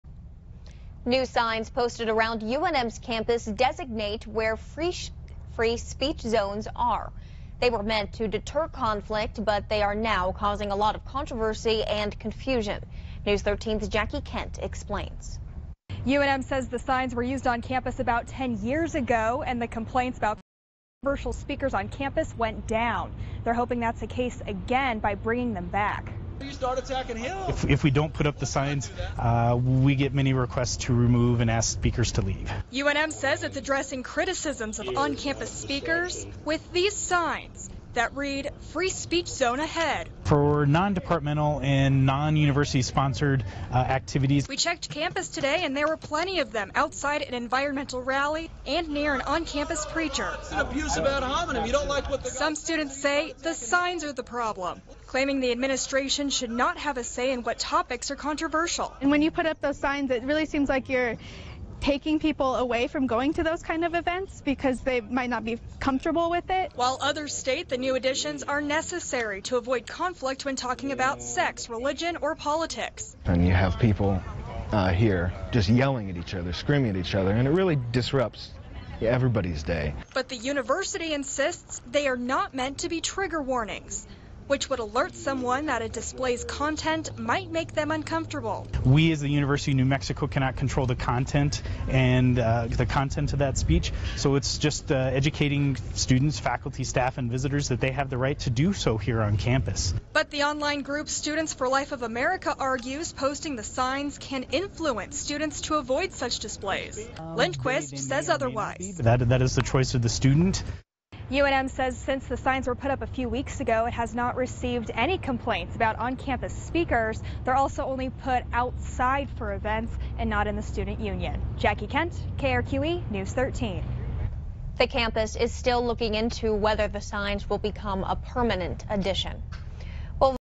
University of New Mexico Has Illegal Trigger Warnings
The University of New Mexico has put up illegal trigger warnings around campus, including around Students for Life displays.